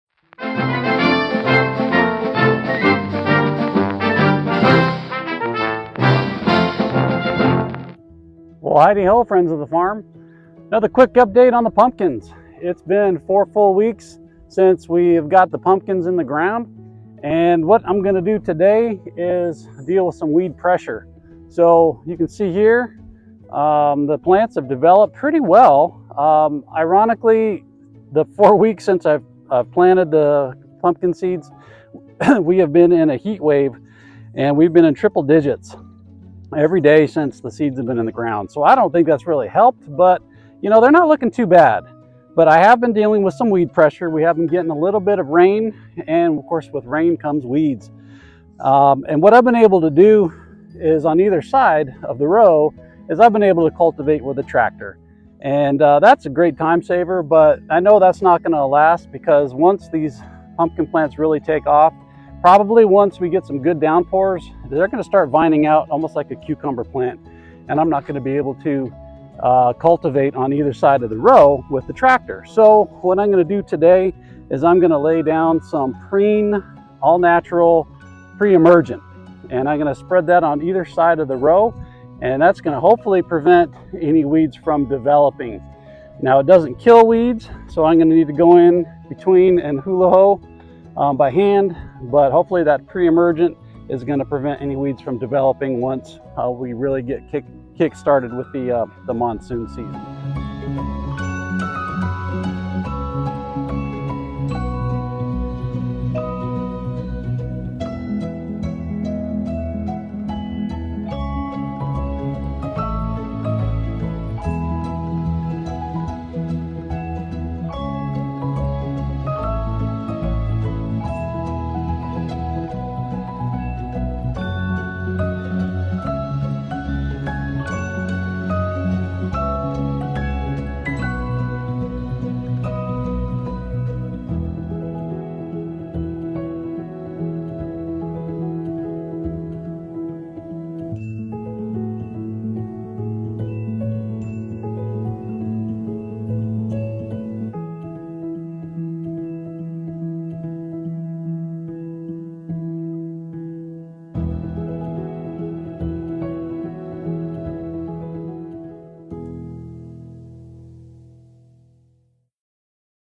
Pumpkin Update | Weed Pressure
4 weeks after pumpkins were planted, addressing some weed pressure.